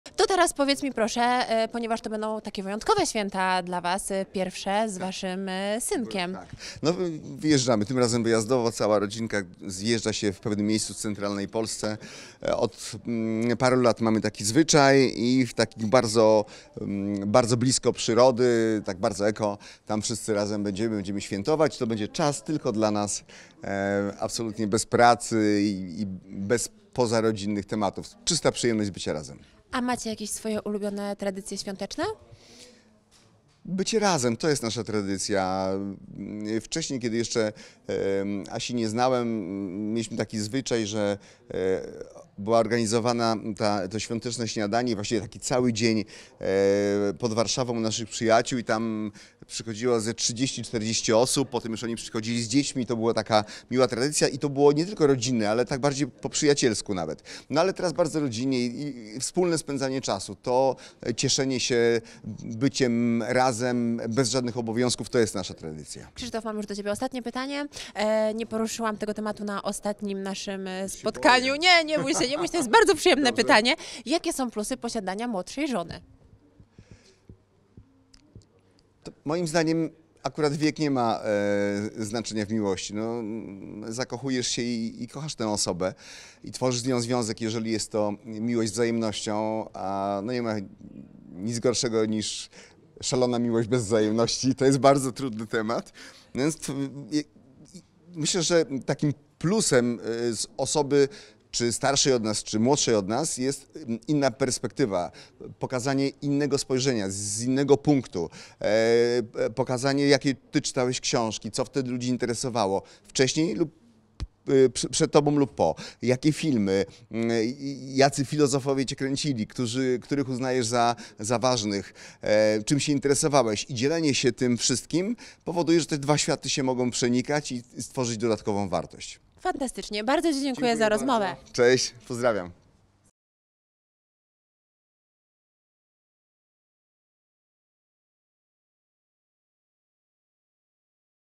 Krzysztof Ibisz o dużej różnicy wieku w małżeństwie. Opowiedział o plusach posiadania młodszej żony
Krzysztof Ibisz opowiedział przed naszą kamerą jak spędzi Wielkanoc. Zapytaliśmy też o dużą różnicę wieku w małżeństwie. Opowiedział o plusach takiego stanu. Szczere wyznanie!